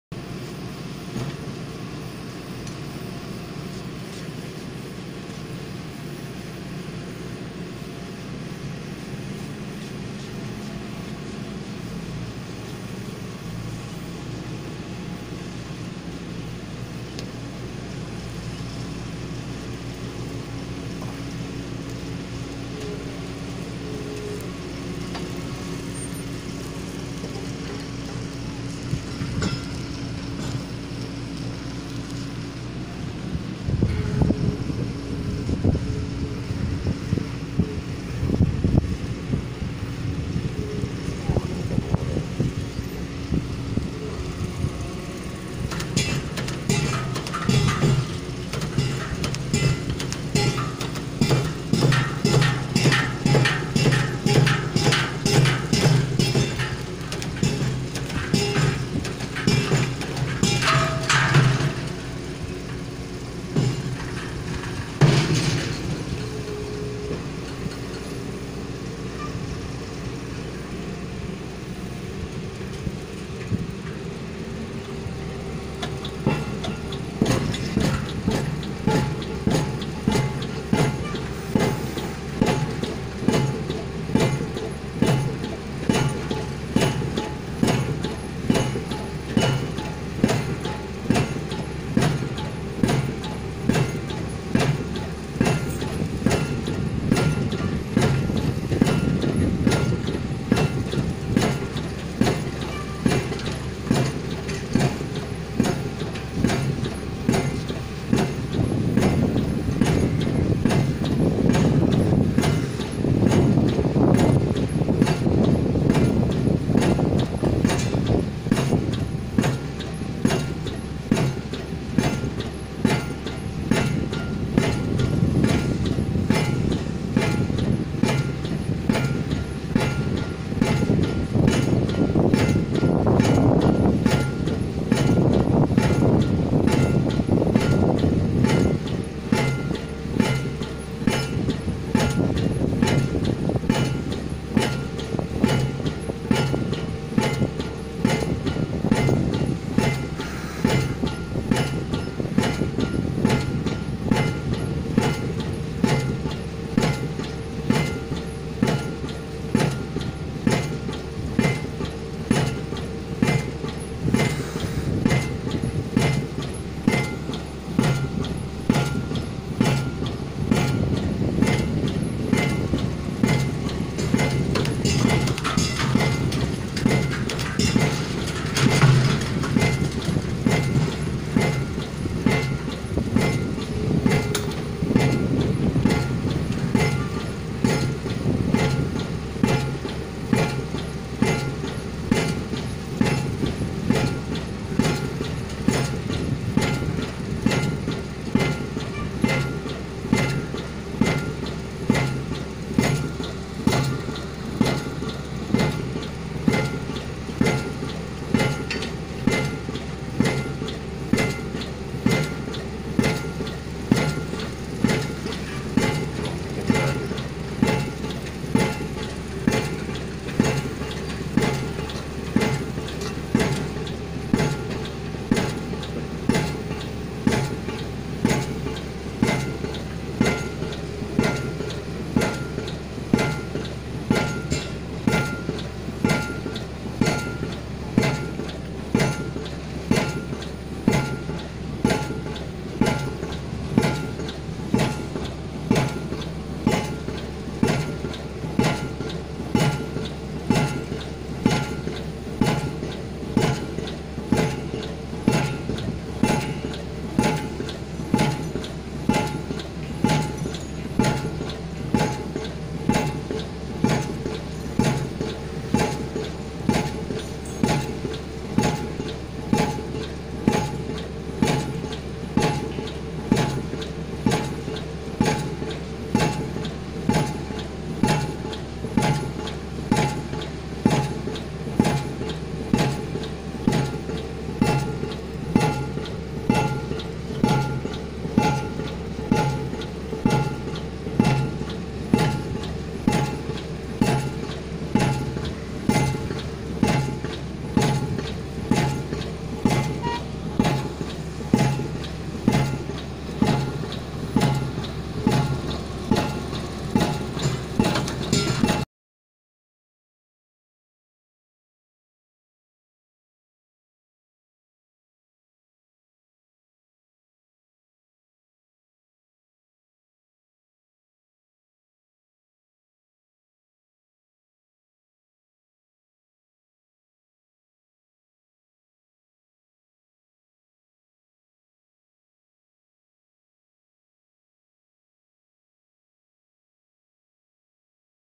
Buat Perumahan Komplek Pakai paku Bumi perumahan komplek
Perumahan yang tahan pada getaran Gempa. Tidak mudah roboh Dan retak.